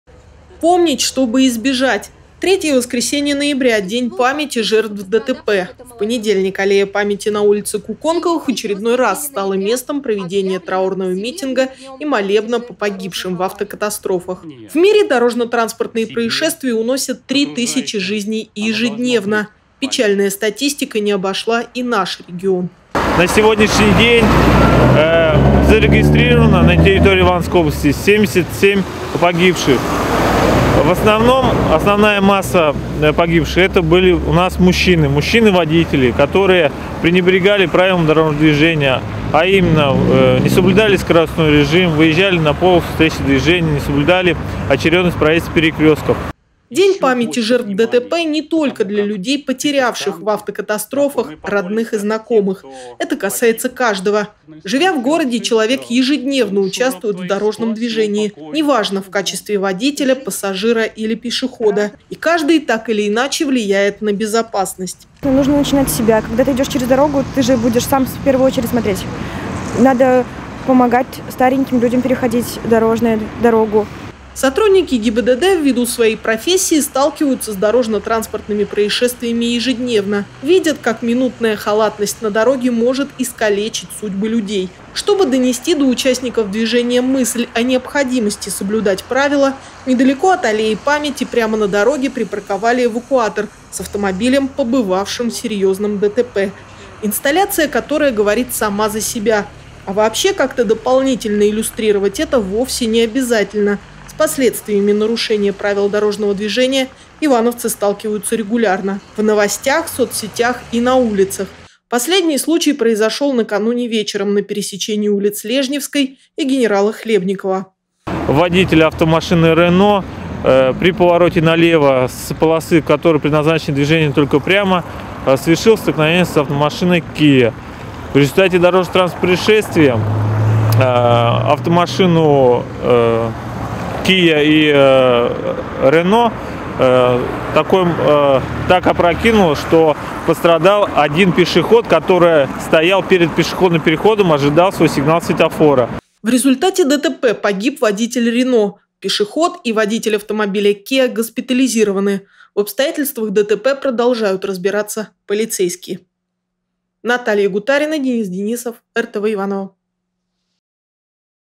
Смертельное ДТП в день памяти жертв ДТП
Помнить, чтобы избежать. Третье воскресенье ноября – день памяти жертв ДТП. В понедельник аллея памяти на улице Куконковых в очередной раз стала местом проведения траурного митинга и молебна по погибшим в автокатастрофах.